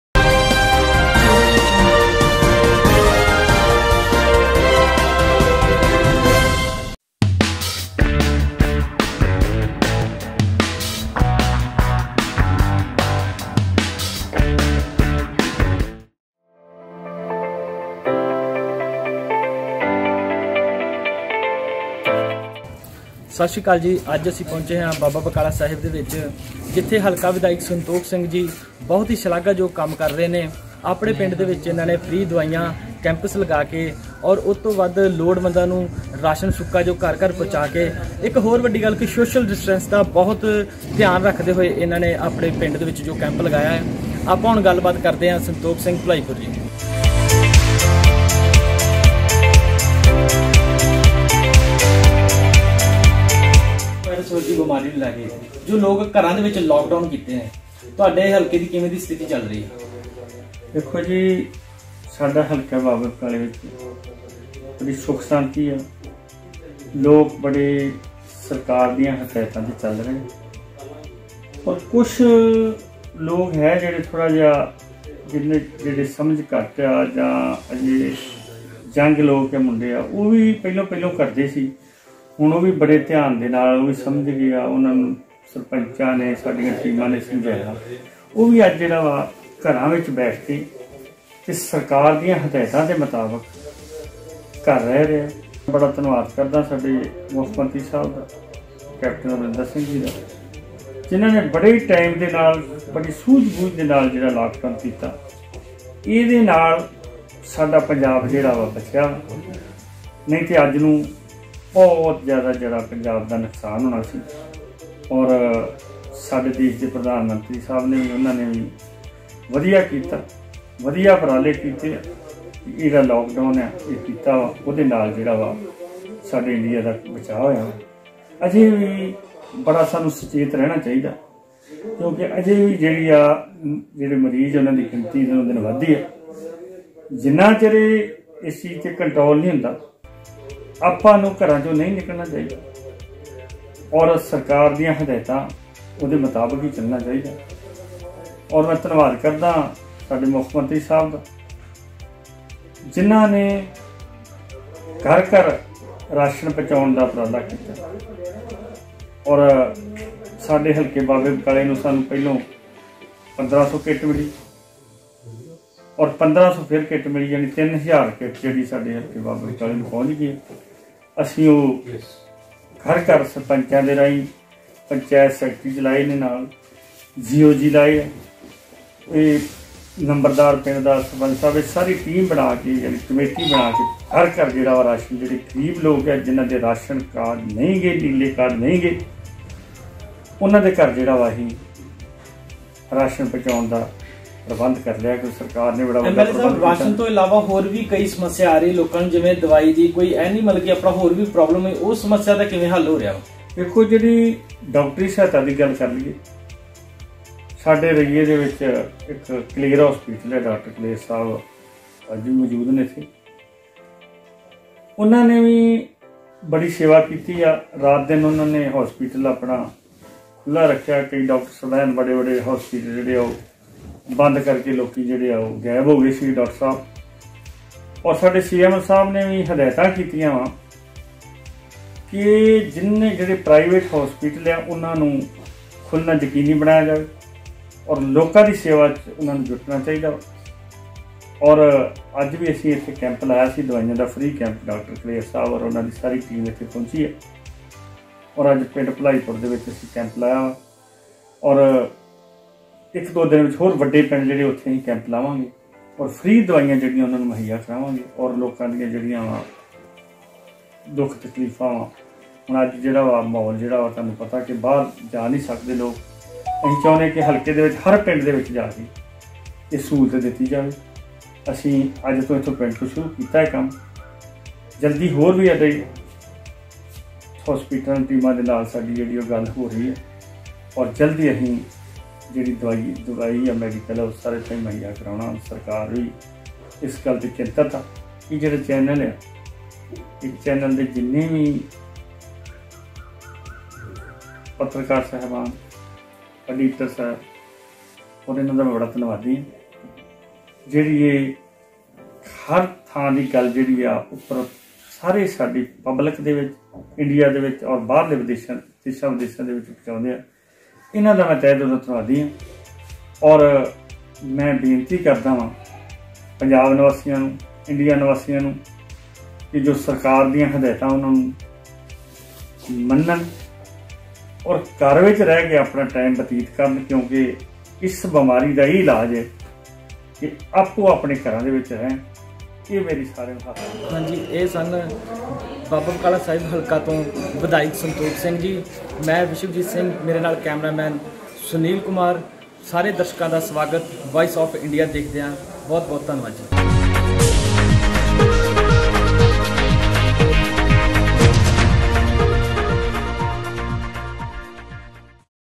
Special conversation with Santokh Singh, MLA of Baba Bakala during Lockdown
Special conversation with Santokh Singh, MLA of Baba Bakala during Lockdown/ ਲੋਕਡੌਨ ਦੇ ਦੌਰਾਨ ਬਾਬਾ ਬਕਾਲਾ ਦੇ MLA ਸੰਤੋਖ ਸਿੰਘ ਦੇ ਨਾਲ ਵਿਸ਼ੇਸ਼ ਗੱਲਬਾਤ